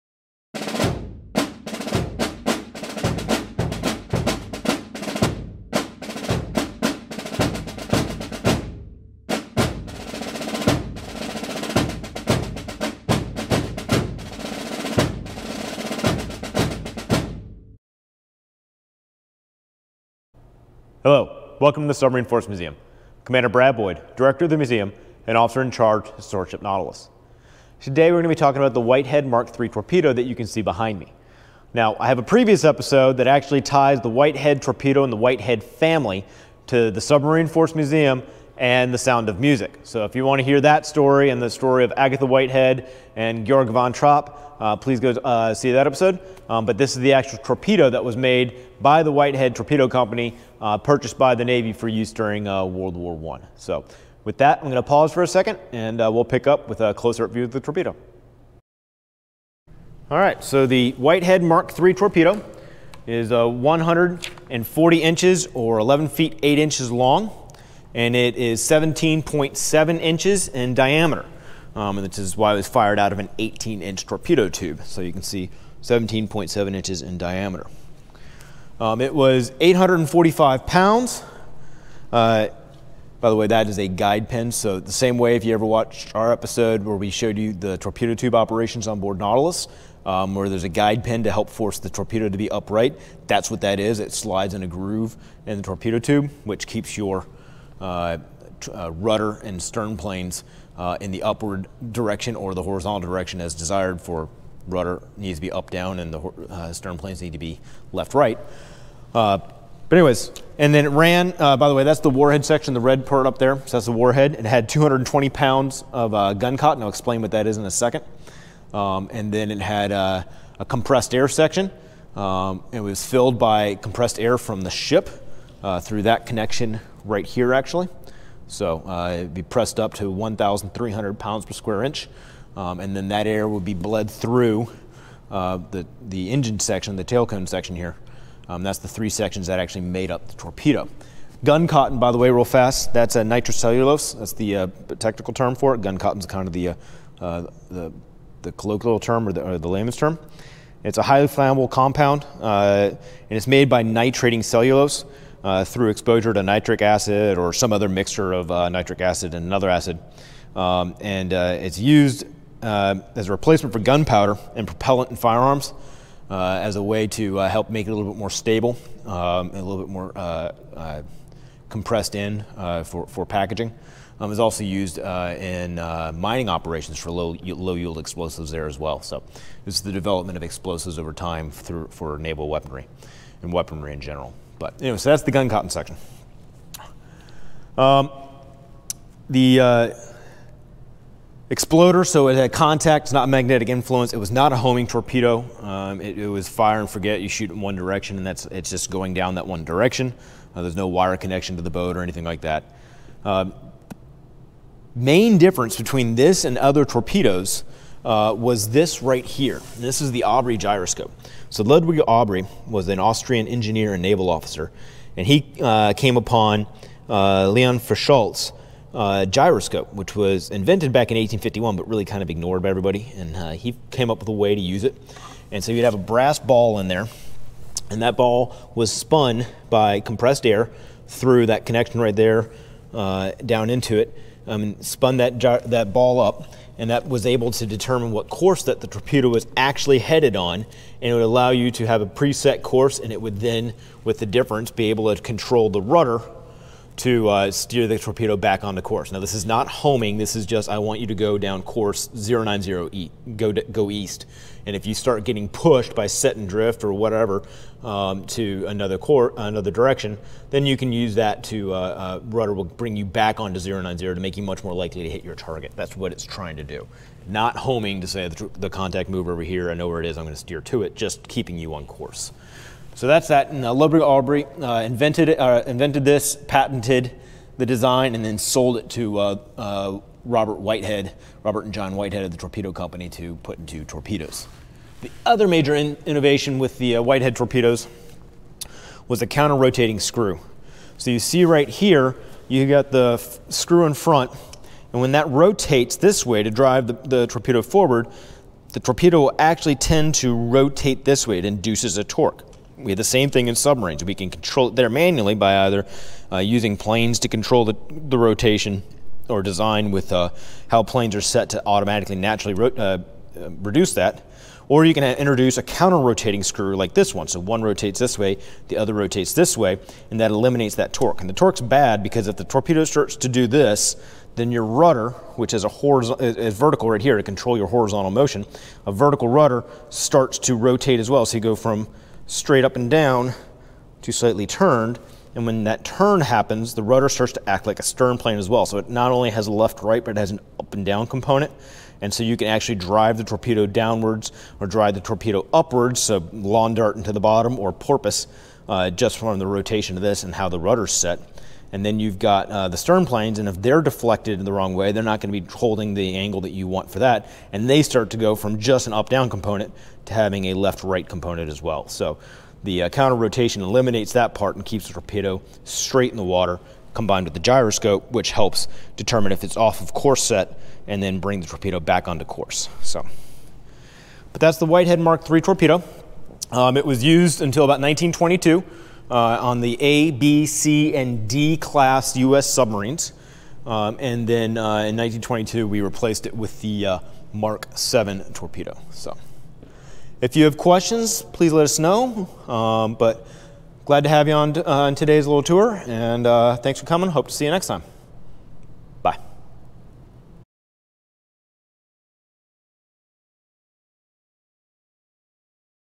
Whitehead MK 3 Torpedo
Come learn about the Whitehead MK-3 Torpedo. Invented by John and Robert Whitehead and one of 100 sold to the U.S. Navy for use in WWI. This torpedo was revolutionary in that it was the first torpedo to use a gyroscope to maintain the torpedo's course (not homing - just maintain a set path).